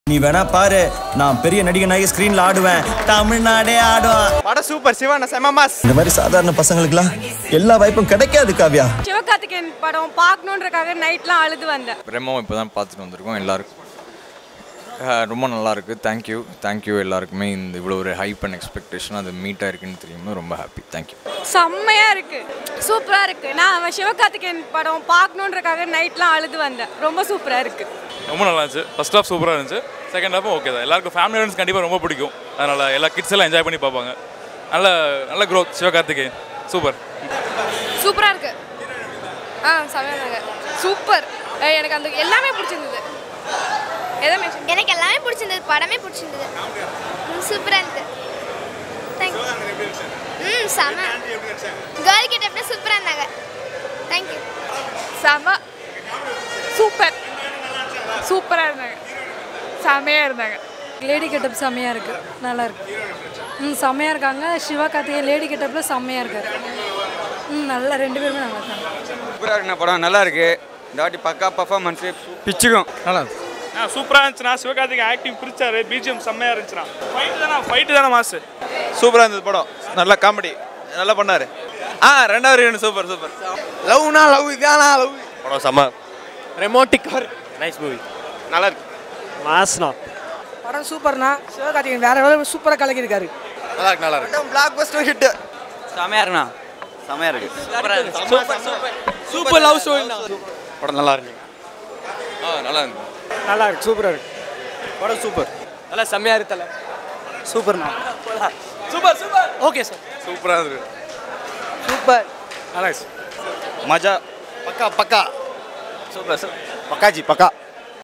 Remo Public Review | Sivakarthikeyan, Keerthy Suresh | Remo Movie Review
Watch Remo Public Review

Watch the public review of Sivakarthhikeyan starring REMO
directed by Bakkkiyaraj Kannan.